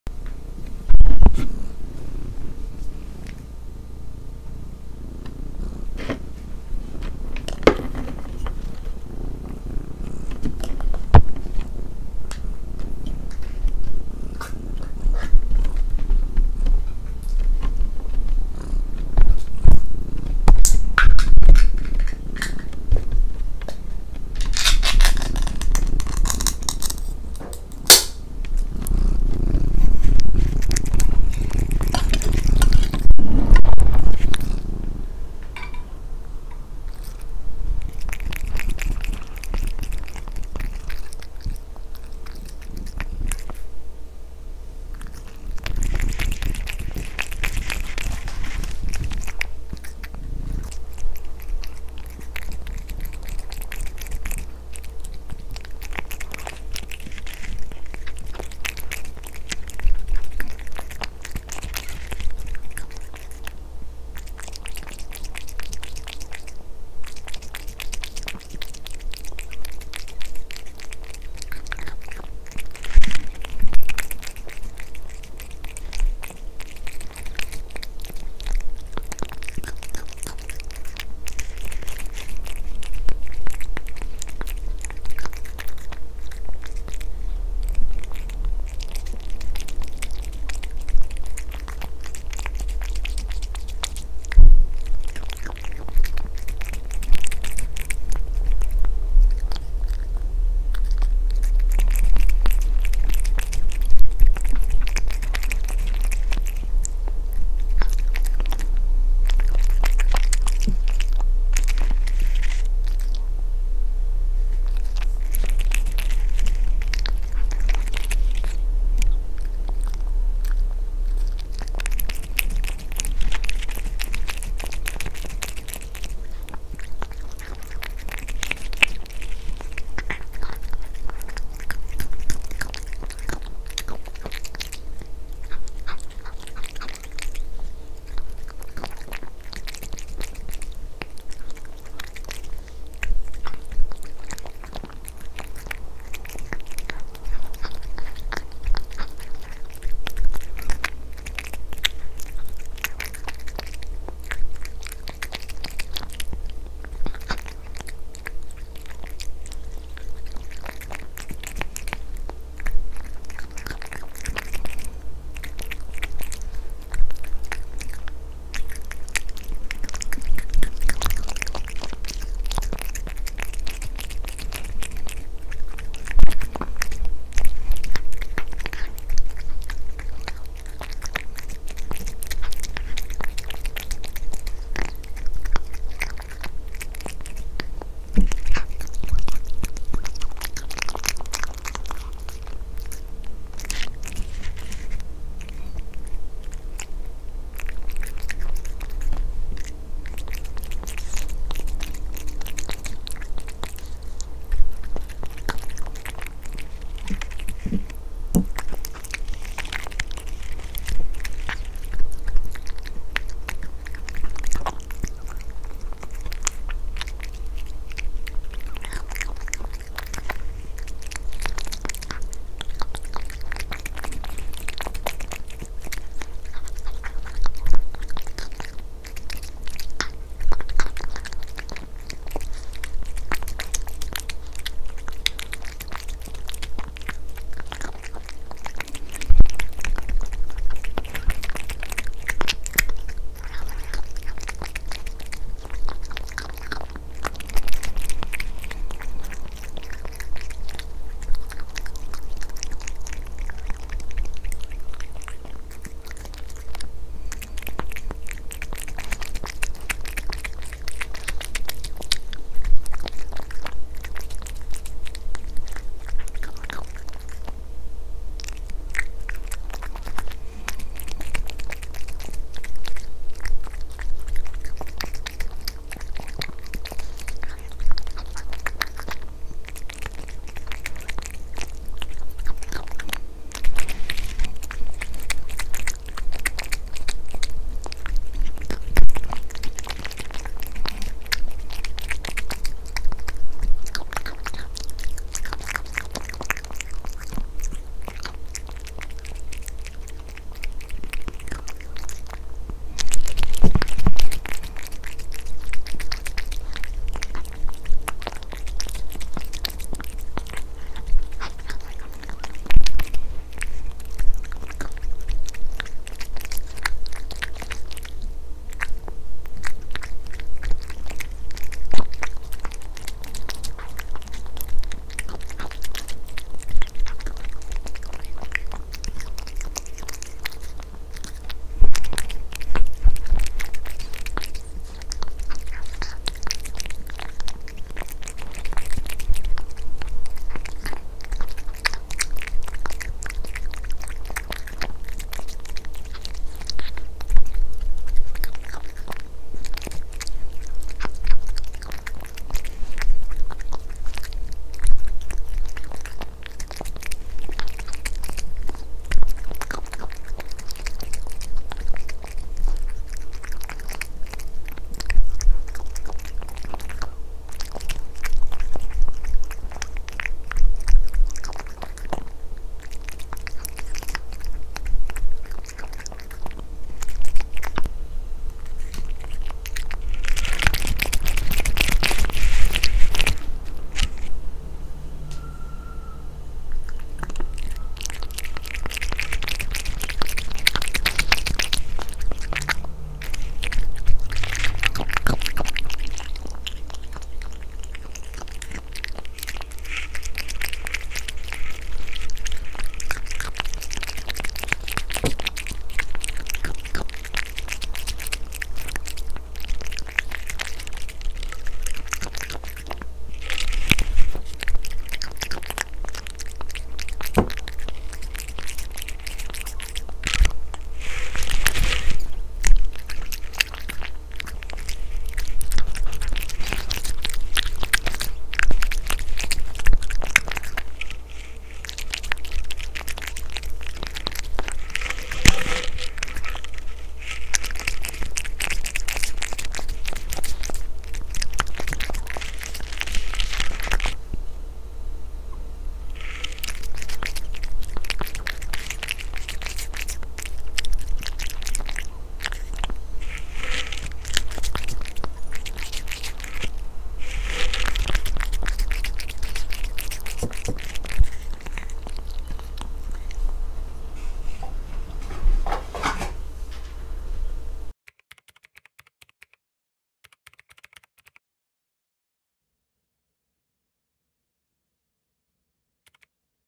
【猫ASMR】猫がチキンを食べる咀嚼音 ゴロゴロ音 くちゃくちゃ音
名前：チェル

猫種：スコティッシュストレート

性別：男の子♂

誕生日：2020/07/24生まれ